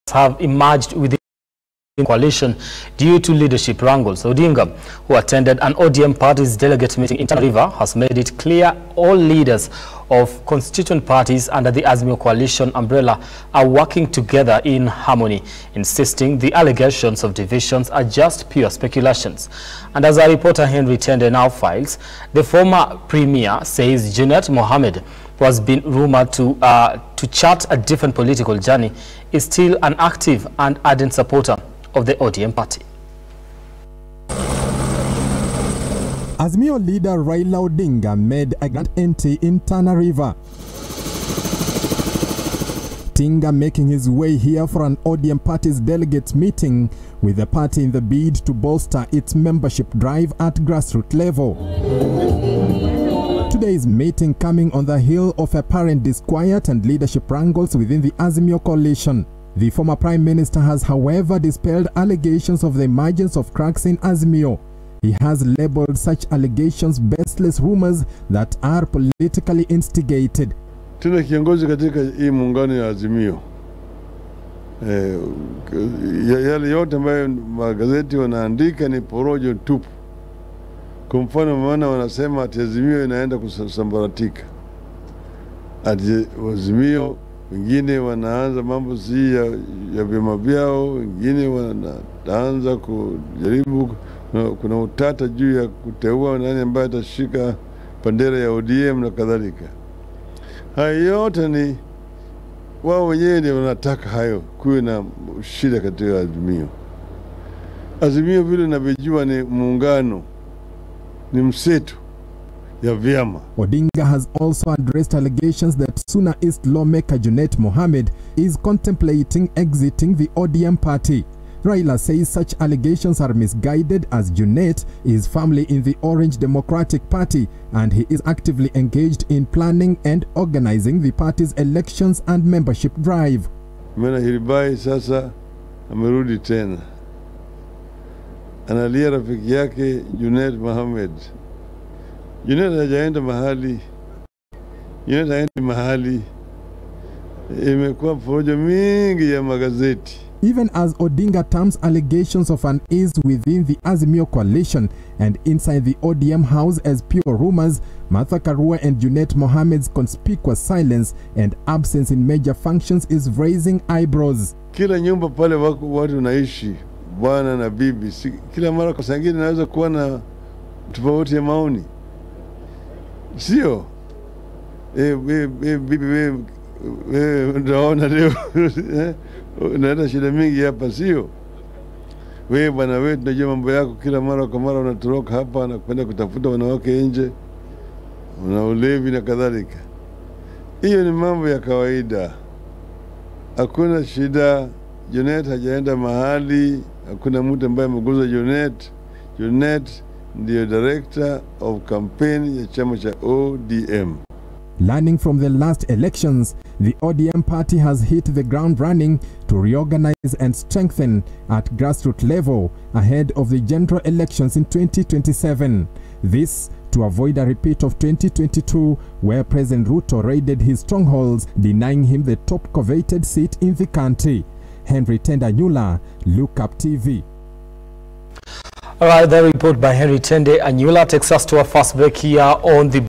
Raila Odinga Rubbishes Claims Of Cracks Within Azimio
Raila: Azimio Still Intact

Opposition Leader Raila Odinga Has Moved To Dispel Rumours That Cracks Have Emerged Within The Azimio Coalition Due To Leadership Wrangles.